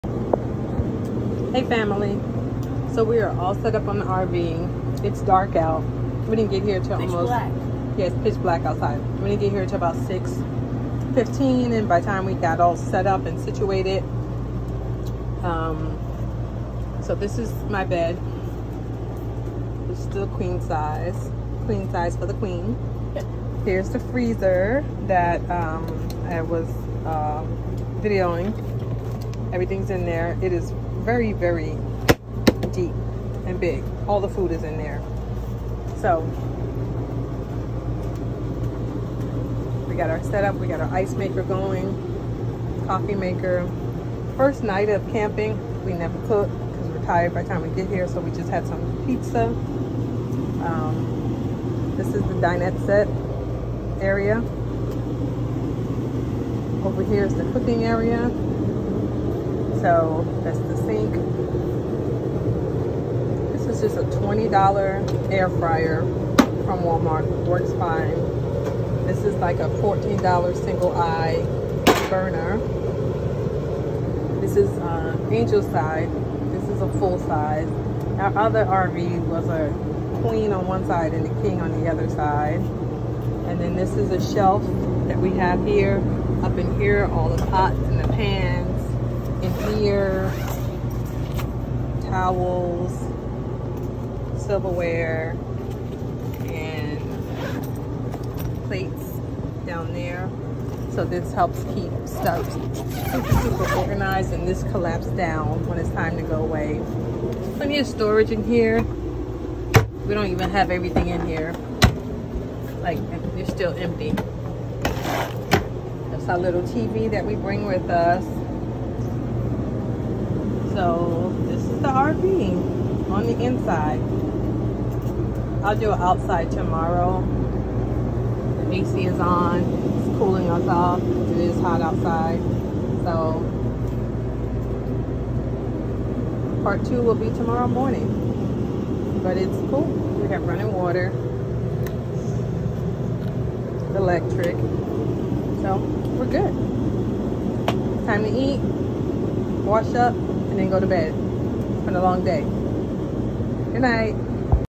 2018 Rockwood Freedom pop up inside tour
2018 Rockwood Freedom pop up tour of inside. Basic set up of our new to us pop up rv.